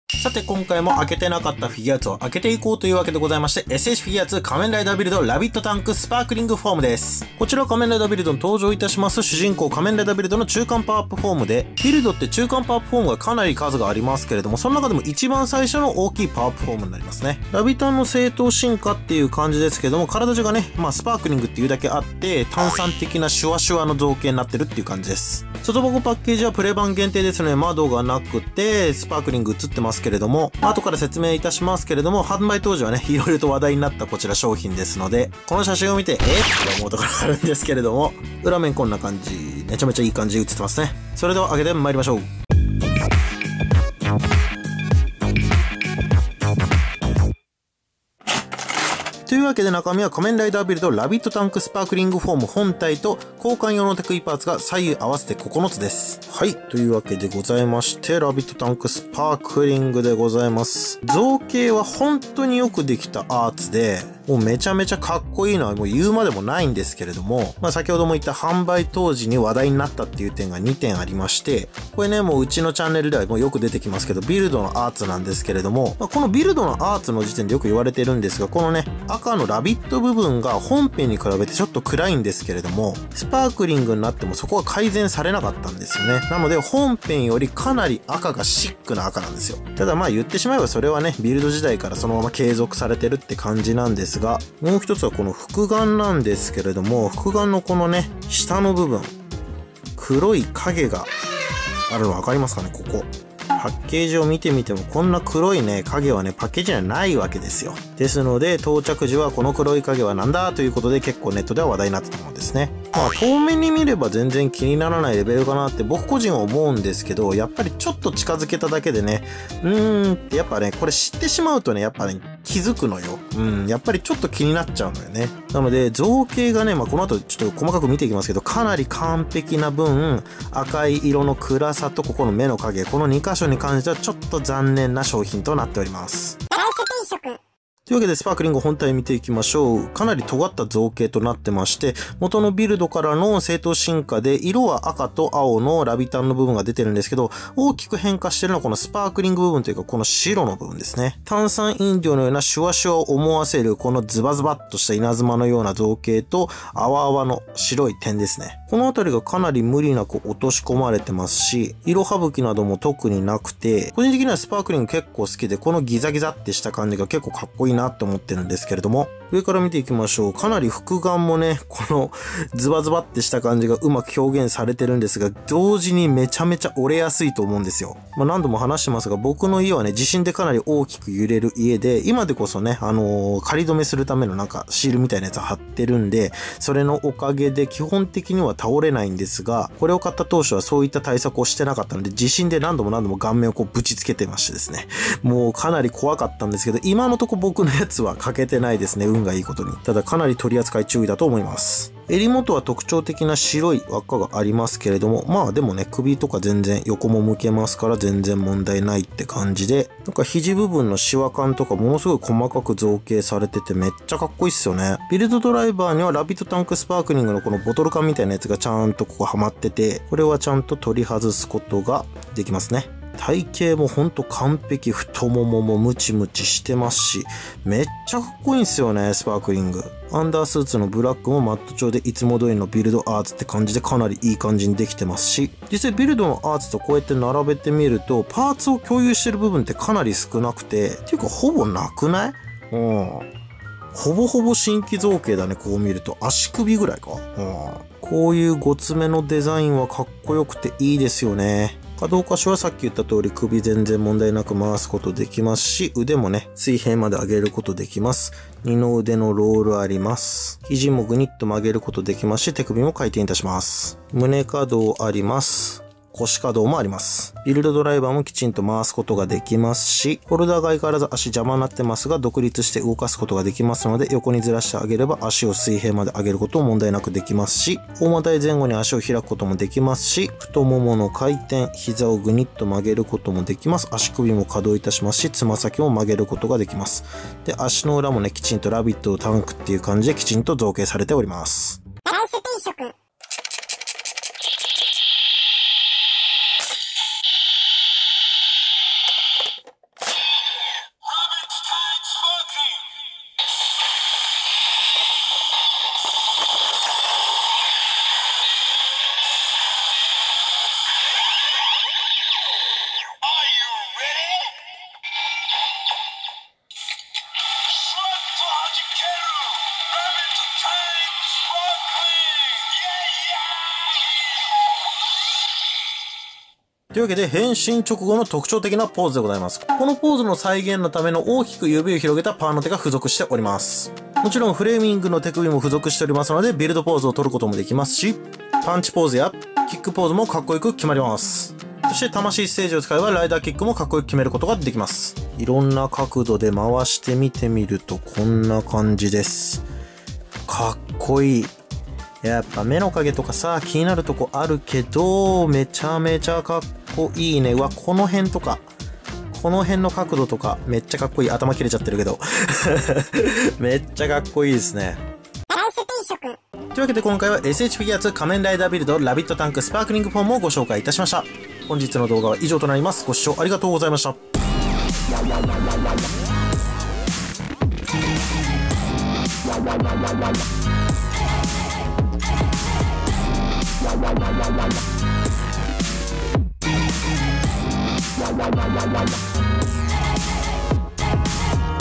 S.H.Figuarts 仮面ライダービルド ラビットタンクスパークリングフォーム ビルドの最初のパワーアップフォーム！しかし複眼と色味が…レビュー！
ライス定食です。

■再生リスト■
玩具レビュー